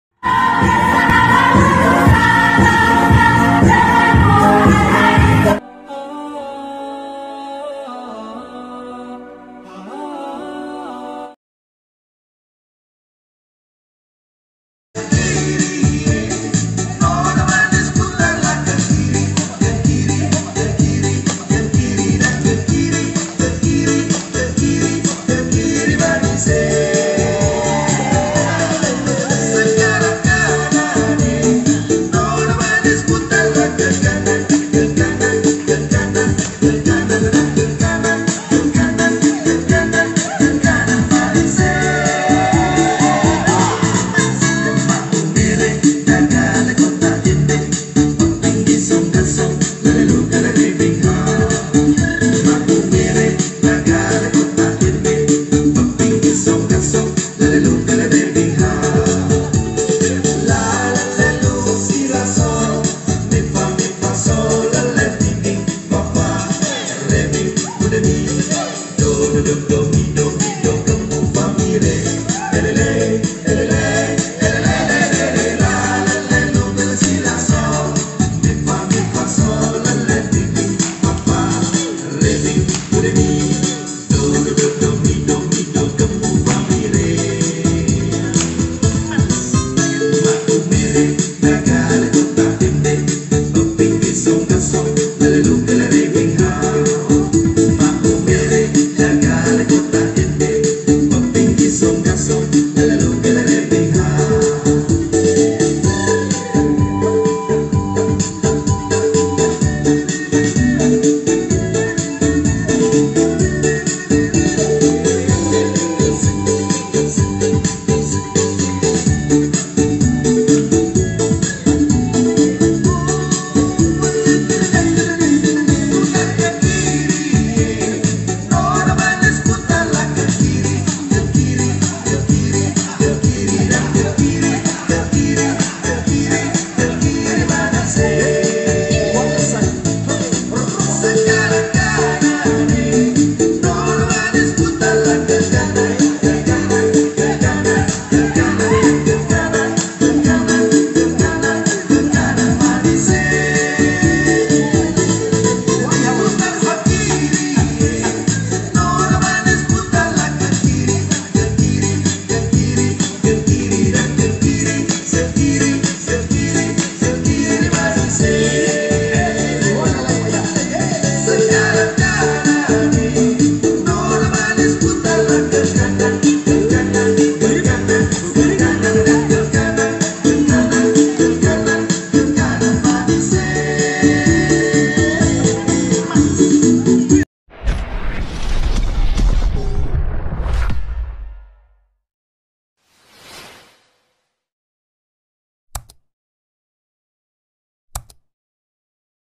Senam Qobla Tahajjud, dilanjutkan dengan Ngaji Qobla Subuh. | Aktivitas 1 Jam Sebelum Waktu Subuh
Aktivitas anak anak kita 1 Jam Sebelum Shubuh, Semoga mereka semua di berikan kekuatan, kesehatan dan keistiqomahan dalam mengawali proses perjuangan sebagai penuntut Ilmu di Hikmatusysyarief, yang mungkin tdk biasa di lakukan ketika di rumah.